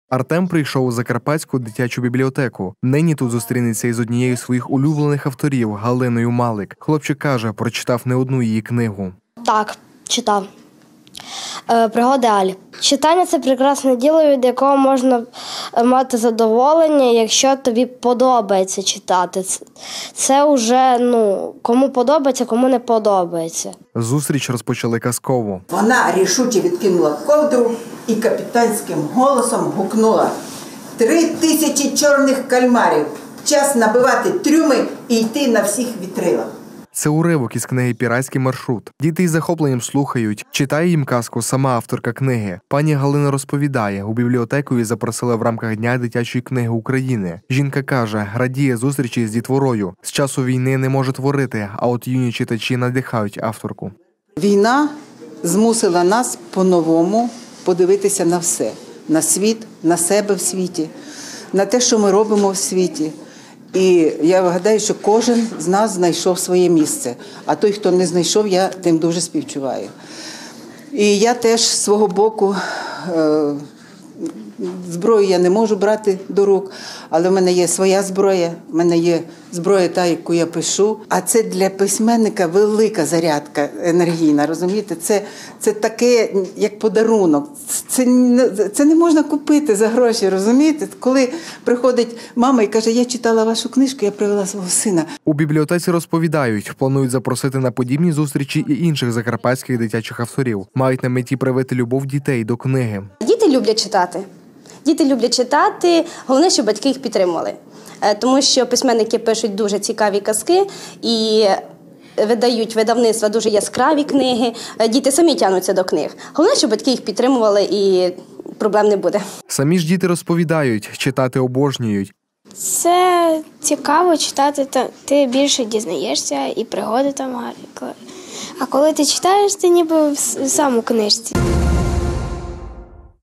День дитячої книги України
Закарпатські автори дитячих книг, просять президента оголосити 30 січня – днем дитячої книги України. Відповідне звернення вже відправили гаранту. А самі в Ужгороді стартували святкувати. Так у кобзарі розпочався тиждень дитячого автора за прилавком, а от в обласну дитячу бібліотеку на зустріч із юними кумирами прийшла відома українська письменниця. Артем прийшов у закарпатську дитячу бібліотеку. Нині тут зустрінеться із однією із своїх улюблених авторів – Галиною Малик. Хлопчик каже прочитав не одну її книгу. Зустріч розпочали казково. Це уривок із книги Пірацький маршрут. Діти із захопленням слухають. Читає їм казку сама авторка книги. Пані Галина розповідає у бібліотеку її запросили в рамках Дня дитячої книги України. Жінка каже, радіє зустрічі із дітворою. З часу війни не може творити, а от юні читачі надихають авторку. У бібліотеці розповідають, планують запросити на подібні зустрічі і інших закарпатських дитячих авторів. Мають на меті привити любов дітей до книги. Самі ж діти розповідають, читати обожнюють. І радять до цієї справи братися всім: і малим і дорослим.